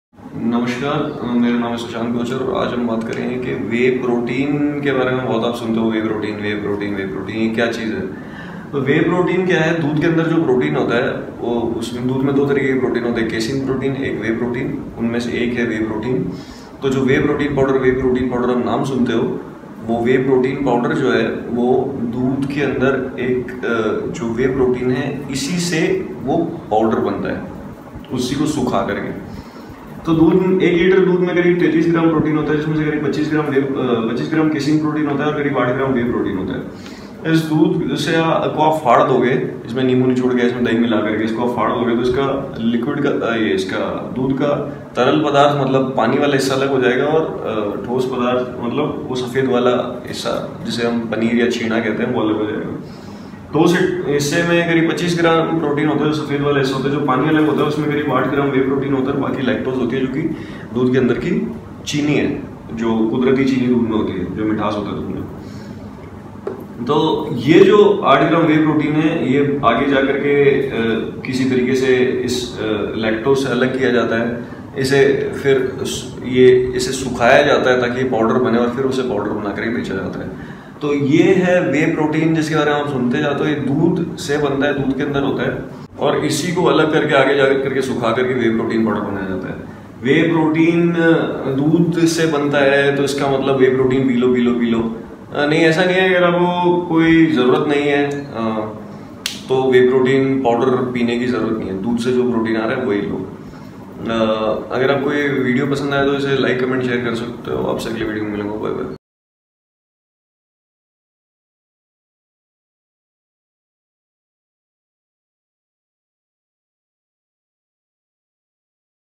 Whey Protein क्या होता है - दूध में ? What is Whey Protein (found in Milk) ? (Hindi)
दूध में Whey Protein क्या होता है  ? Video tells what the Whey Protein found in Milk is.